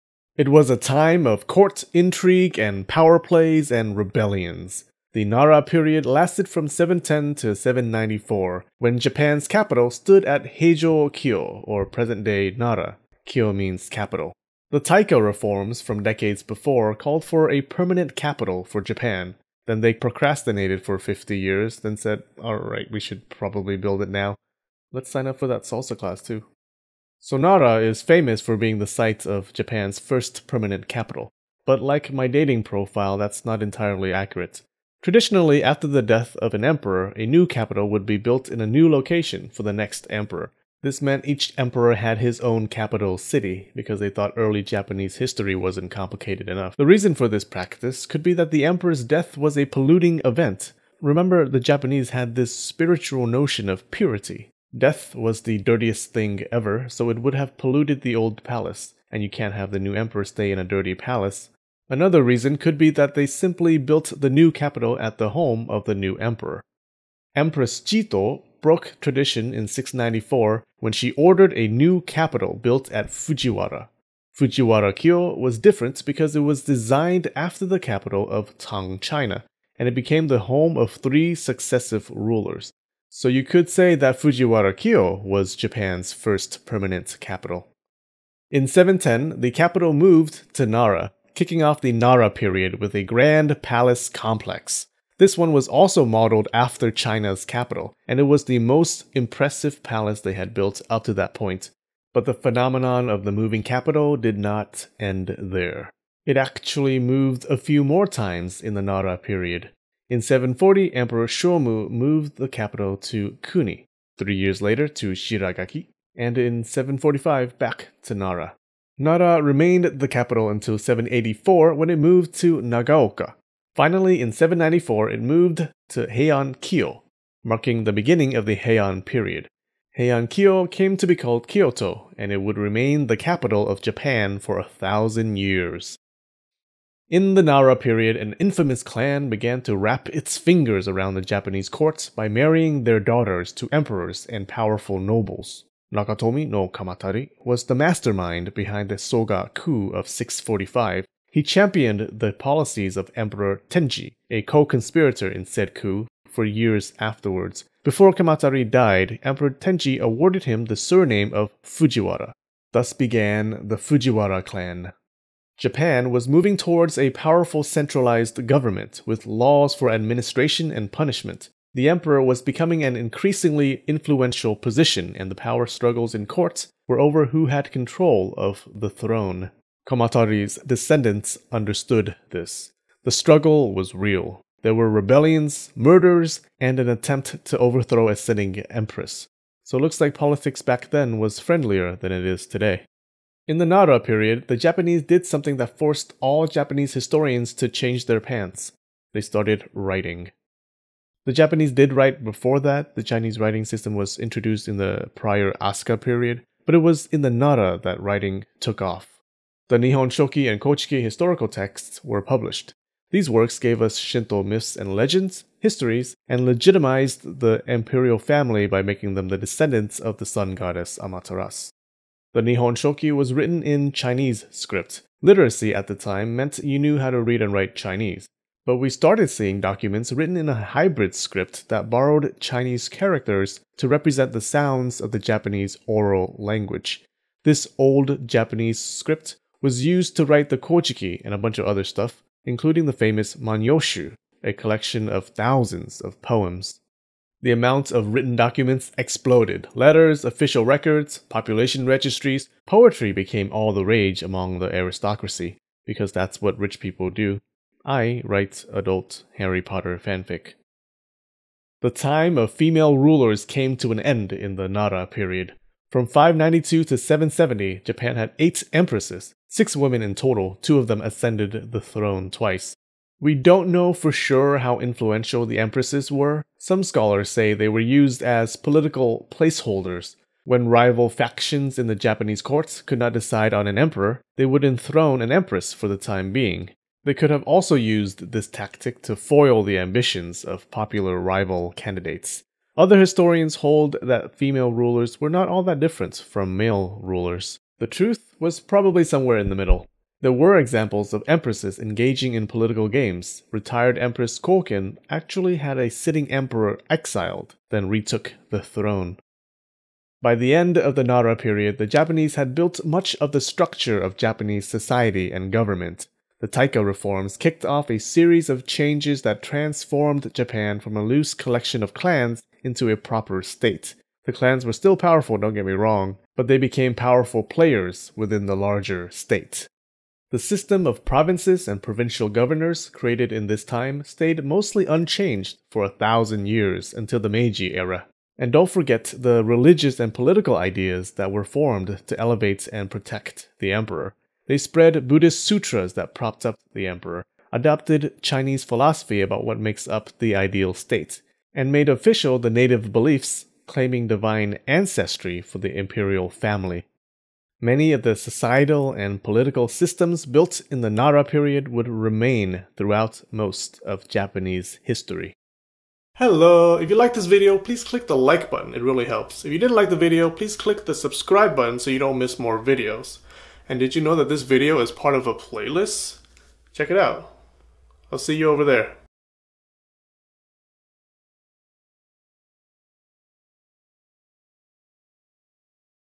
The Nara Period | History of Japan 27
An overview of Japan's Nara Period (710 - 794).

(you get a history blog and posts about random stuff)

(shirts, phone cases, stickers, and more!)

➤➤ SOURCES
1. Premodern Japan
2. Japan Emerging
3. Cambridge History of Japan
4. A History of Japan: Revised Edition